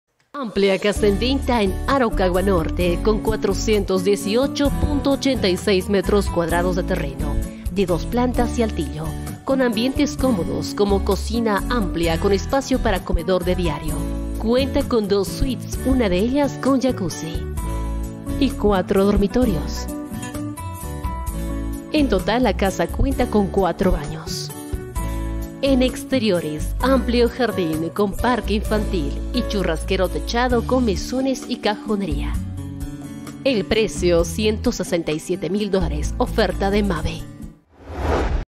AMPLIA CASA EN ESQUINA A LA VENTA, COCHABAMBA, EN ZONA AROCAGUA NORTE
A la altura del km 5 Av. Circunvalación, Sup. Lote 418,86 m2, Sup. Construida 196,97 m2, de dos plantas más altillo: consta de 2 suites, 4 dormitorios amplios, 4 baños, escritorio, sala amplia, cocina, con garaje para 3 vehículos, lavandería, los servicios básicos instalados Gas domiciliario e internet, con tanque de 8000 l, árboles frutales, con excelente acceso a transporte público. Se acepta financiamiento bancario,  *Precio $us 167.000.-*